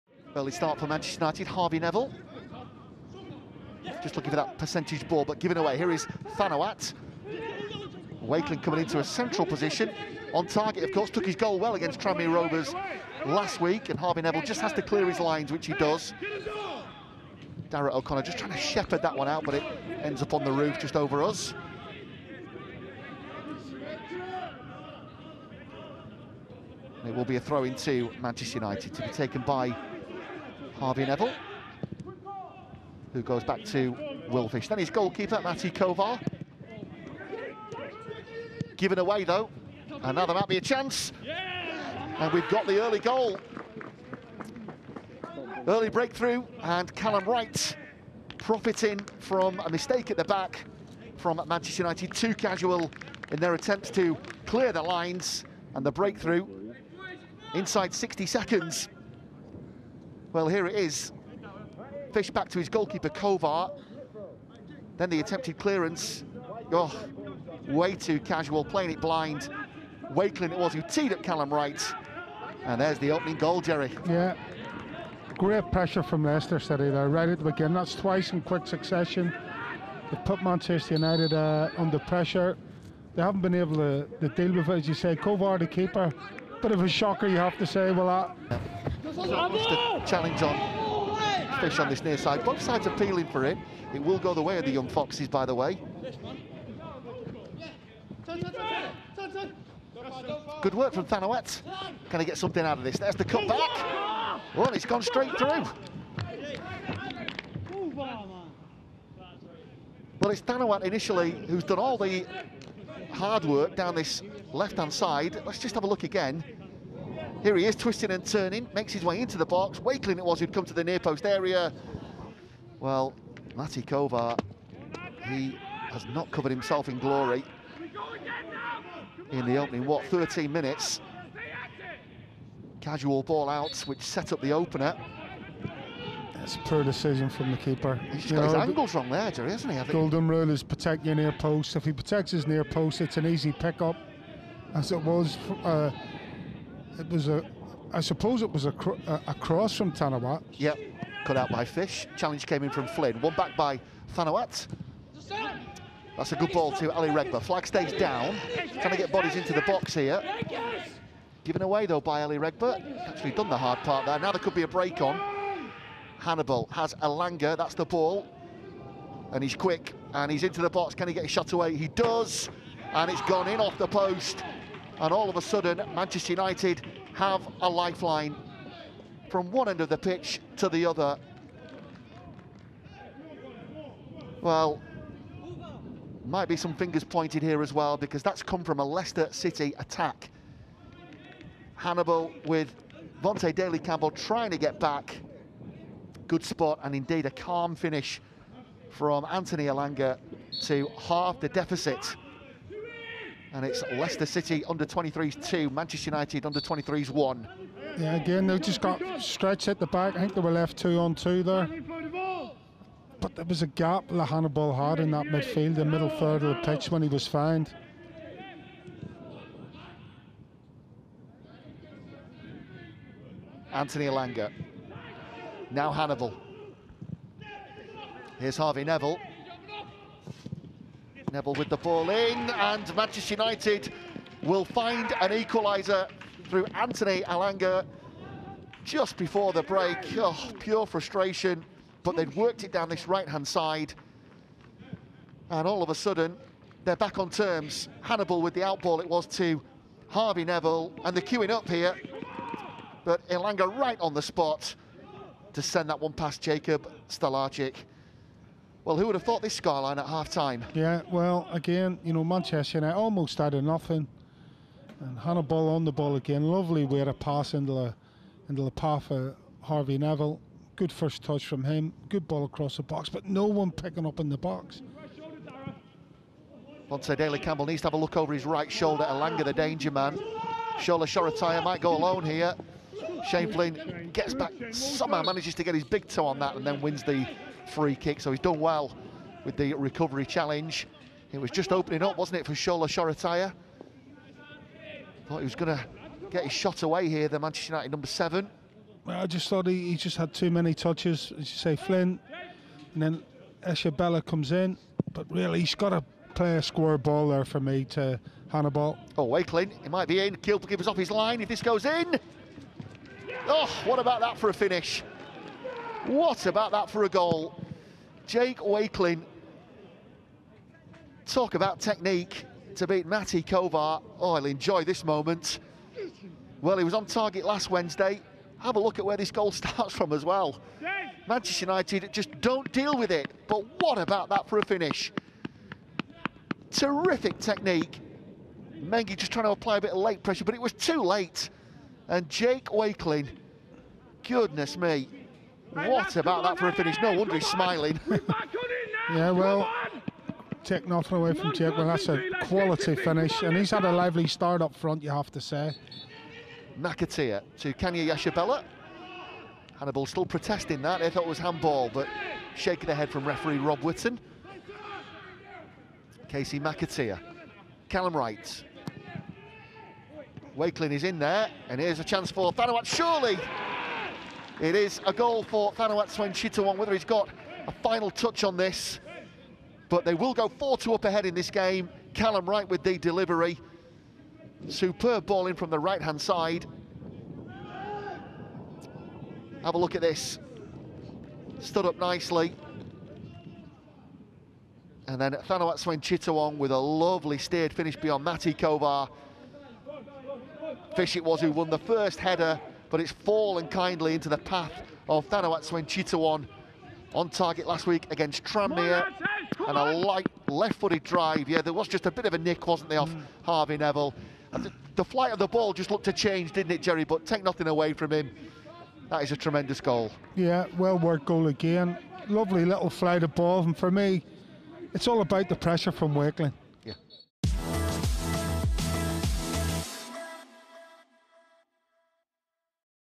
Leicester City U23s 4 Manchester United U23s 2 | Premier League 2
The Foxes hit four in their first Premier League 2 Division 1 fixture at LCFC Training Ground on Monday evening.

A double from Thanawat Suengchitthawon and further goals from Callum Wright and Jake Wakeling secured three points and kicked off 2021 in style for the young Foxes.